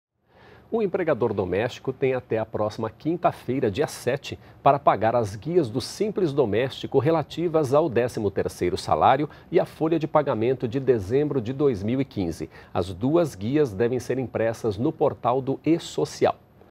Guias do eSocial devem ser pagas até a próxima quinta-feira
NBR NOTÍCIAS – 05.01.16: As guias do Simples Doméstico referentes ao 13º salário e a folha de pagamento do mês de dezembro de 2015 devem ser pagas pelo empregador até o dia 07 de janeiro. Os documentos já estão disponíveis no portal do eSocial.